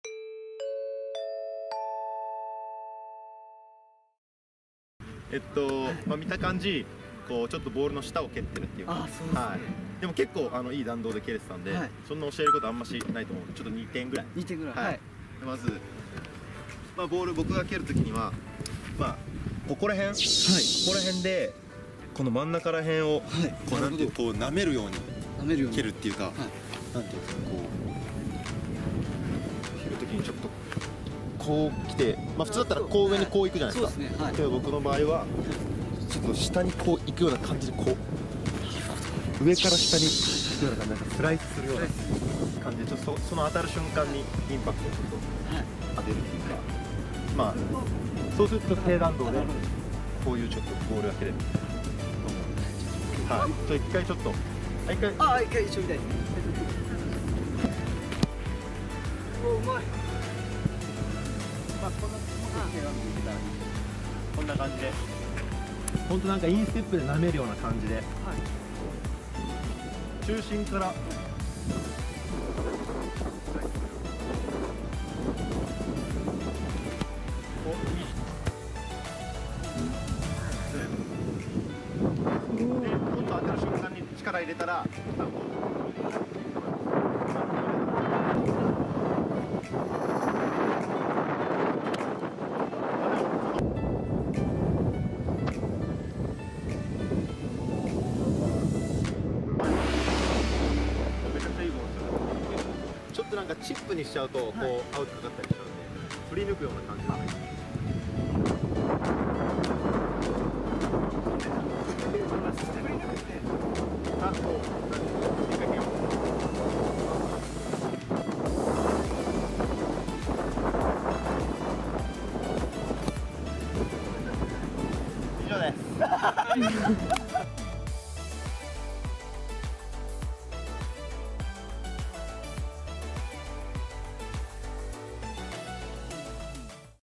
【低弾道キック】現役Jリーガーが教える低弾道キックの極意!!
今回低弾道キックの極意を特別に教えます。
まだみてない方に共有してみてください!!
乞うご期待!
気軽にやってほしい動画などコメントもどしどし待っています！！
乞うご期待！！！
ぜひ最後までお楽しみください！！！